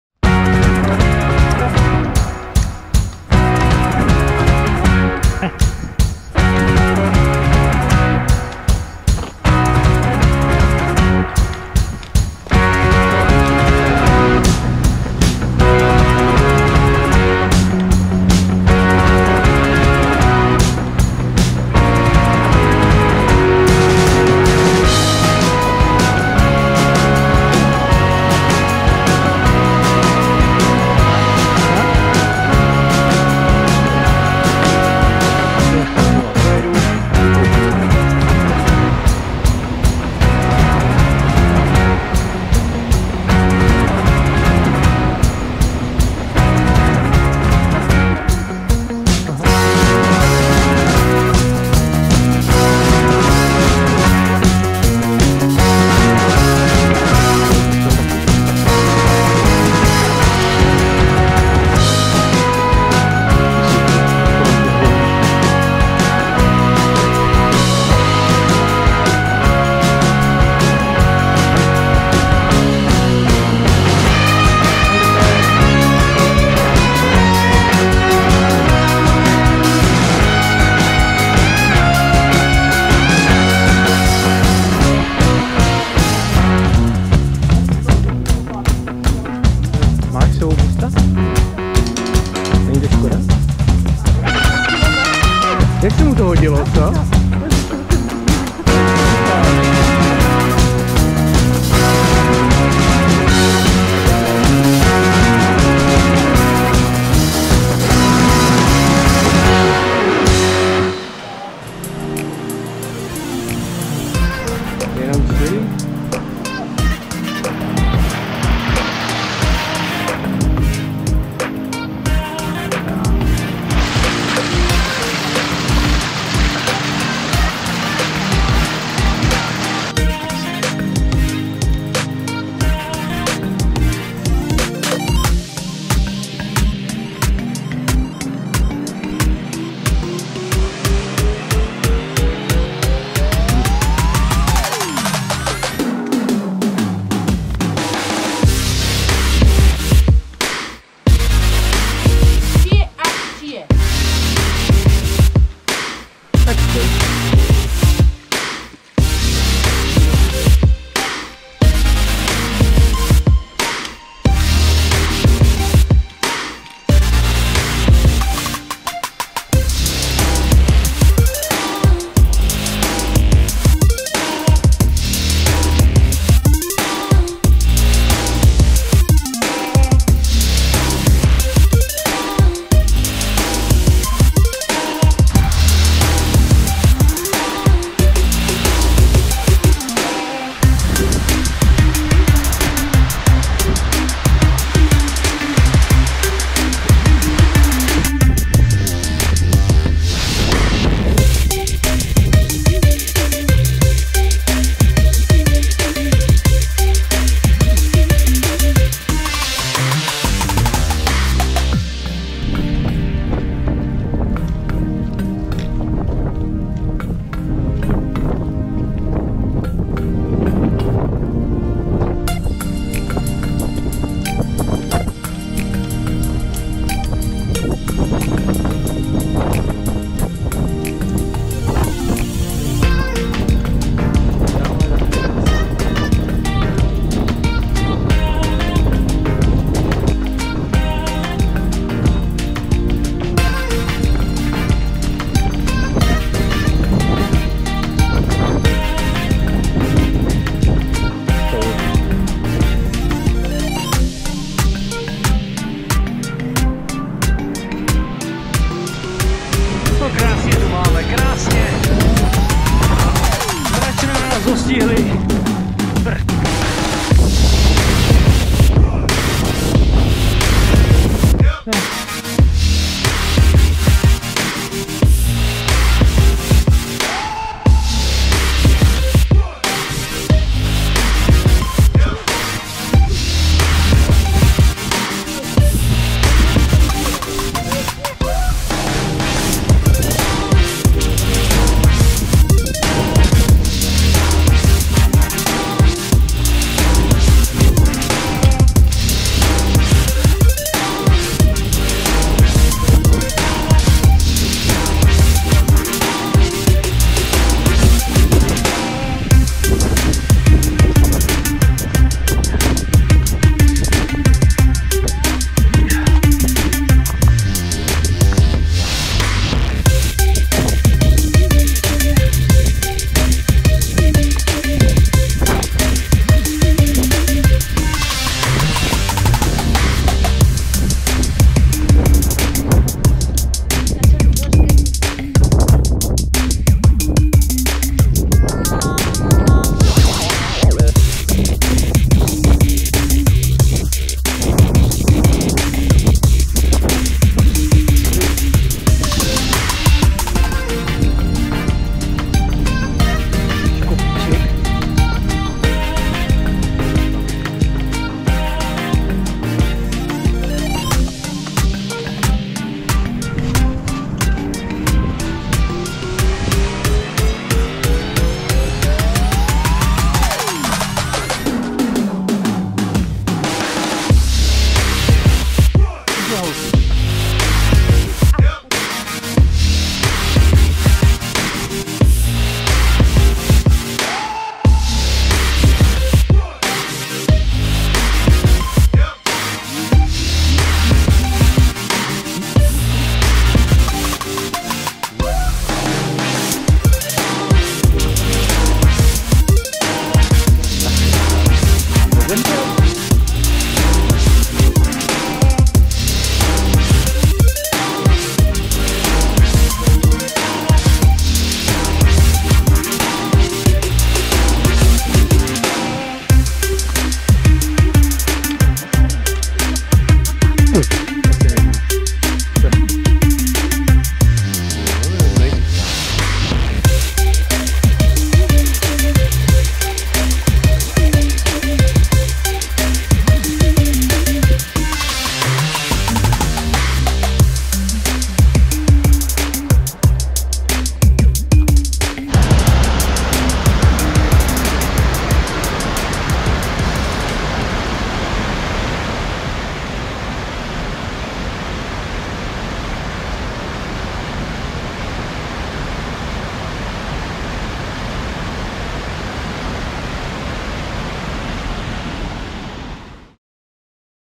Sněžka
Hora Sněžka je se svými 1603 metry nejvyšší horou nejen Krkonoš, ale i celé České republiky. Skalnatým vrcholem hory Sněžky, který má rozlohu asi 30 akrů, prochází česko-polská hranice. Na severní polské straně se svahy Sněžky svažují do údolí řeky Lomničky. Jižní svahy na české straně spadají do Obřího dolu, západní klesají na Úpské rašeliniště a východní přechází v Obří hřeben. Jméno Sněžka vzniklo z původního pojmenování Sněžná, odvozeného od stavu „sněhem pokrytá“. Než se však současné jméno hory v roce 1823 ustálilo, jejím prvním jménem byl Pahrbek Sněžný, Sněžovka a občas se jí říkalo i Sněhovka.